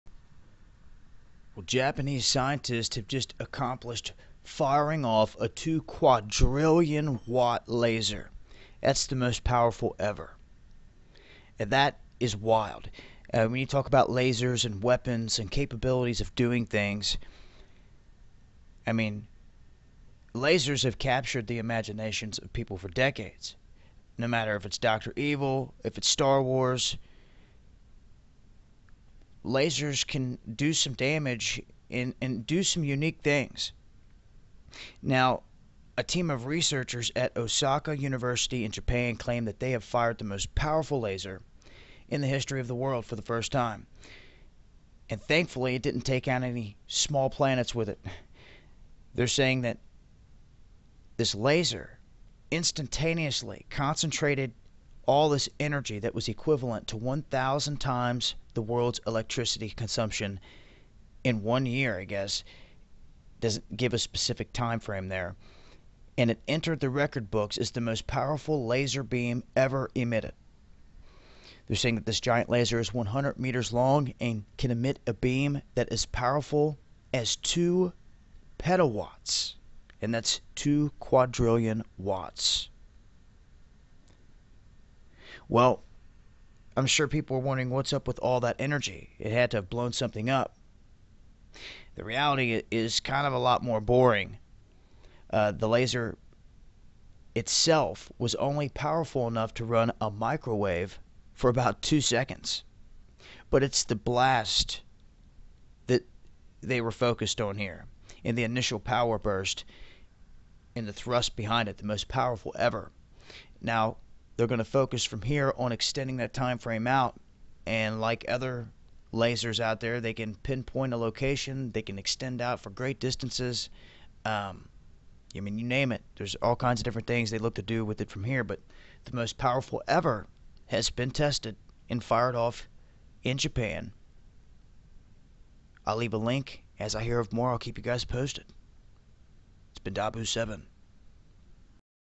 Japanese Scientists Fire A 2 Quadrillion-Watt Laser, The Most Powerful Ever
Lasers. They’ve captured our imaginations for decades and were even at the center of one of Dr. Evil’s most beloved running gags in the Austin Powers movies. And now a team of researchers at Osaka University in Japan claims that they’ve fired the most powerful laser in the history of the world for the first time… and thankfully it didn’t take out any small planets with it.

The researchers say that the laser “instantaneously concentrated energy equivalent to 1,000 times the world’s electricity consumption and entered the record books as the most powerful laser beam ever emitted.” This giant laser is 100 meters long and can emit a beam that’s as powerful as 2 petawatts (i.e., 2 quadrillion watts).

So with all that energy, this massive laser must have blown something up, right? Sadly, the reality was a lot more boring than that.

“The energy of the laser beam itself was only powerful enough to run a microwave for about two seconds,” the researchers explain.